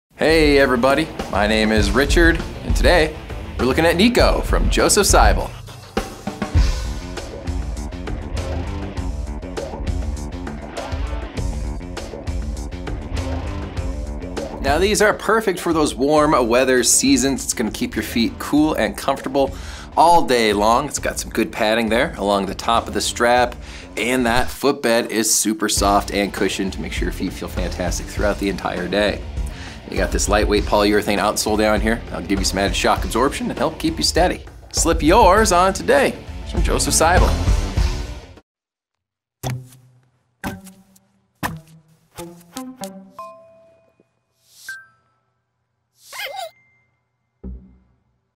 Josef Seibel Nico 01 SKU:8841384
Warm weather style is a breeze with the casual Josef Seibel® Nico 01 sandal!
Easy slip-on silhouette.
Rich and supple leather upper with stitch detail.
Leather lining and leather-covered cushioned footbed for all-day comfort.
Flexible and lightweight PU outsole for long-lasting wear.
Imported.